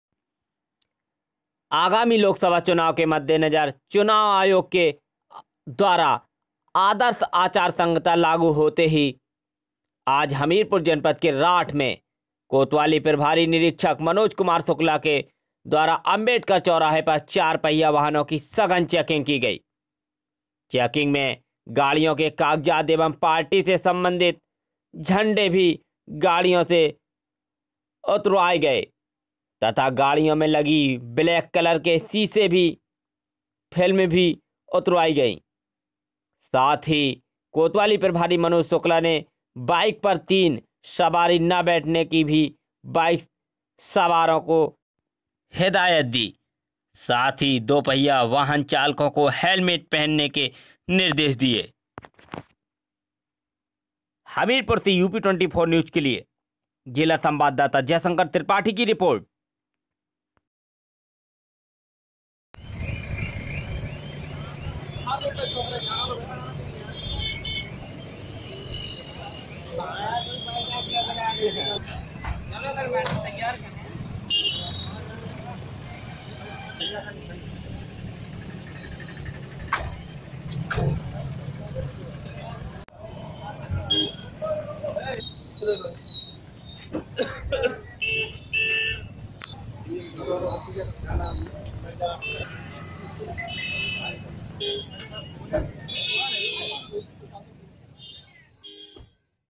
राठ. वाहनों की चैकिंग कर हटवाये काले शीशे, वाहन चालकों मे हड़कंप.up24news
आगामी लोकसभा चुनाव के मद्देनजर चुनाव आयोग के द्वारा आदर्श आचार संहिता लागू होते ही आज हमीरपुर जनपद के राठ मे कोतवाली प्रभारी निरीक्षक मनोज कुमार शुक्ला के द्वारा अम्बेडकर चौराहे पर चार पहिया वाहनों की सघन चेकिंग की गयी . चेकिंग मे गाड़ियों के कागजात एवं पार्टी से संबंधित झंडे भी गाड़ियों से उतरबाये गये तथा कुछ गाड़ियों मे लगी ब्लेक कलर के शीशे की फिल्म भी उतरवाई गई । साथ ही कोतवाली प्रभारी मनोज कुमार शुक्ला ने बाइक पर तीन सवारी न बैठने की भी बाइक सवारों को हिदायत दी । साथ ही दुपहिया वाहन चालकों को हेलमेट पहनने के निर्देश दिये ।

*हमीरपुर से जयशंकर त्रिपाठी की रिपोर्ट*